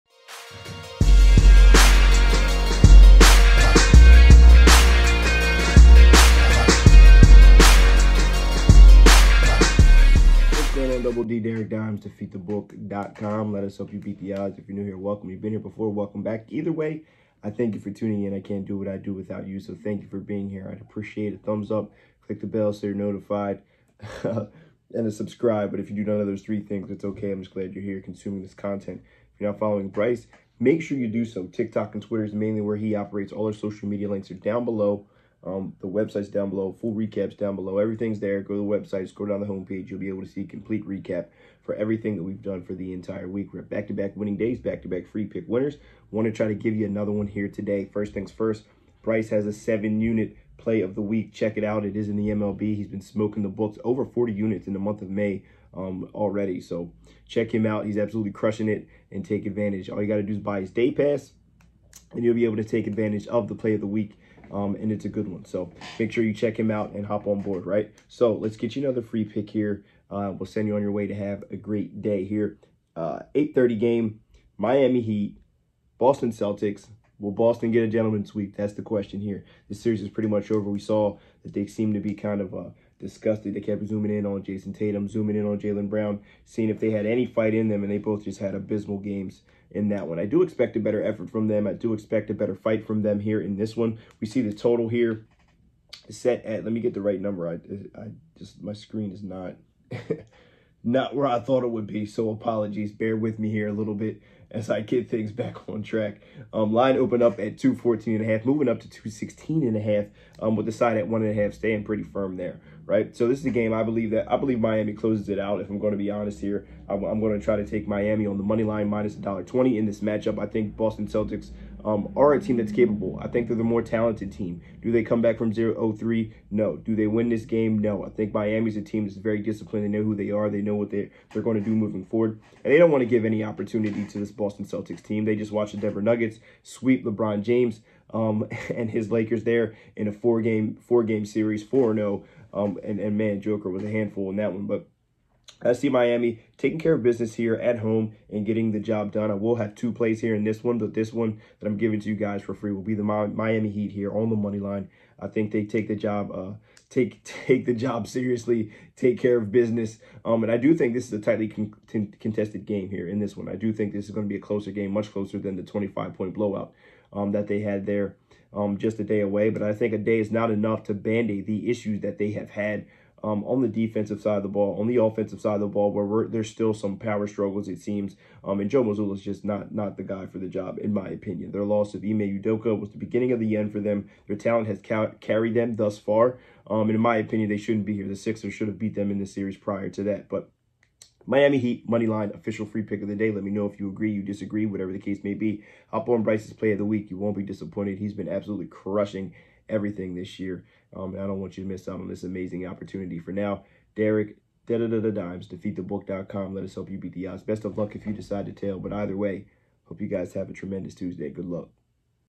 Boston Celtics vs Miami Heat Free Game 4 ECF Predictions 5/23/23
Company Twitter: @Defeat_The_Book
Twitter & Tik Tok: @DerekDimes